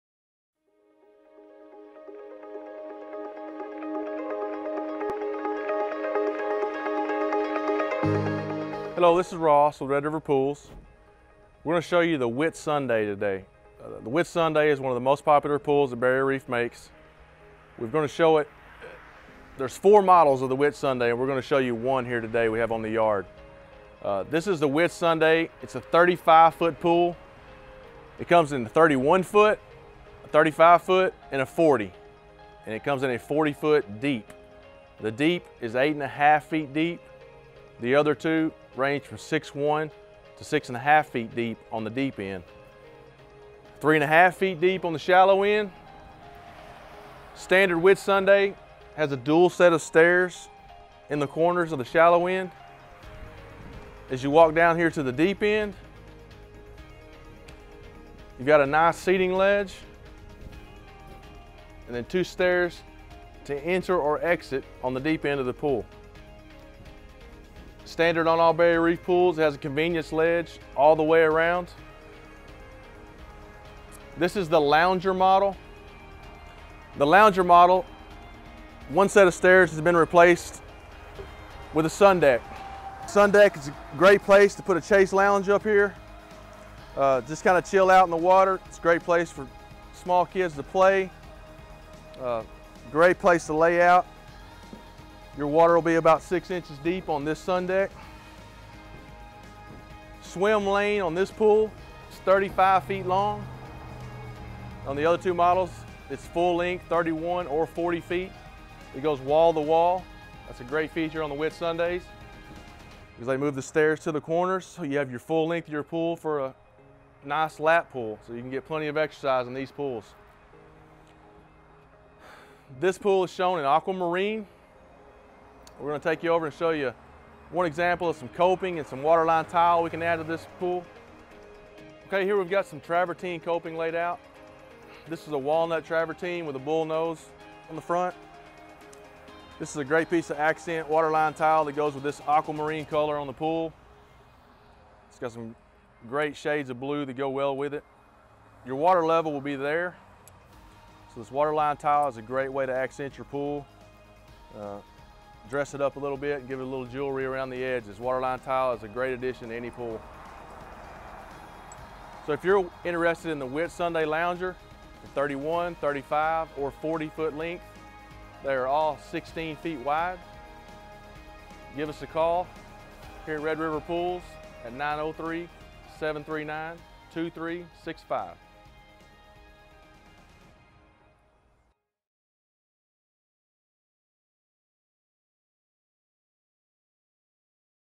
Whitsunday Lounger Barrier Reef Pool || Red River Pools
Learn more about the Whitsunday Lounger Model manufactured by Barrier Reef Pools. Ross talks through the design, specs and options available with this model.